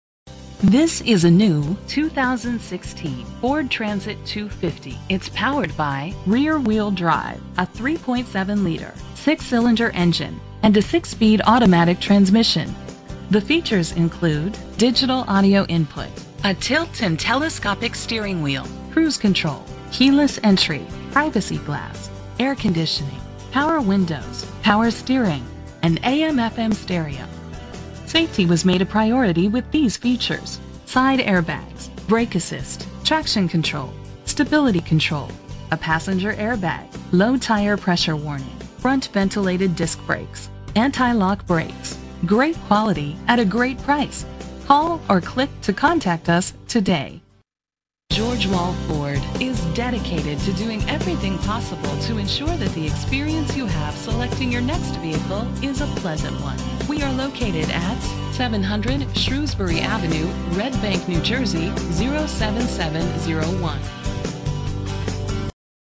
2016 Ford Transit-250 - Red Bank NJ
Phone:
Year: 2016
Make: Ford
Model: Transit-250
Trim: 148 WB Cargo
Engine: 3.7 liter 6 cylinder 24 valve
Transmission: 6-Speed Automatic
Color: Ingot Silver Metallic
Mileage: 0
Address:
700 Shrewsbury Ave.